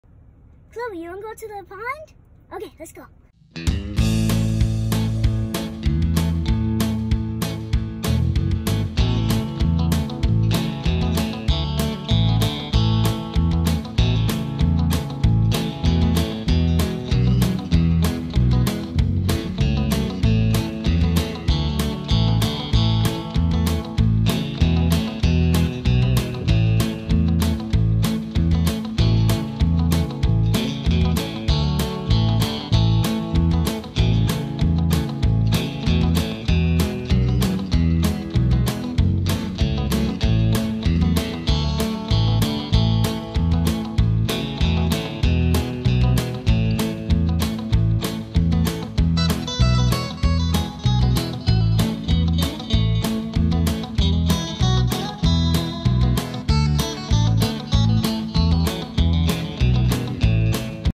💦🦆Going To The Duck Pond🦆💦
Watch Cloe go to the duck pond with her friends, Freya and Saga! 😍